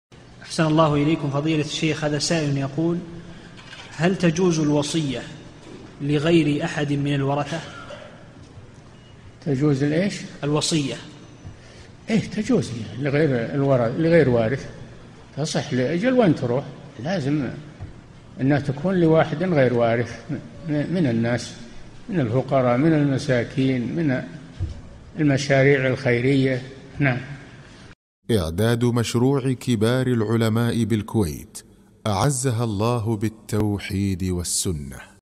[1999 -3022] هل تجوز الوصية لغير أحد من الورثة؟ - الشيخ صالح الفوزان
-"[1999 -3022] السؤال: هل تجوز الوصية لغير أحد من الورثة؟
الجَوَابُ: إيه تجوز لغير الوارث، تصح لأجل وين تروح؟ لازم أنها تكون لواحدٍ غير وارث من الناس من الفقراء من المساكين من المشاريع الخيرية.
للاشتراك في قناة الشيخ صالح الفوزان

للاستفسار والإقتراحات والدعم
رئيس الفريق
لمشروع كبار العلماء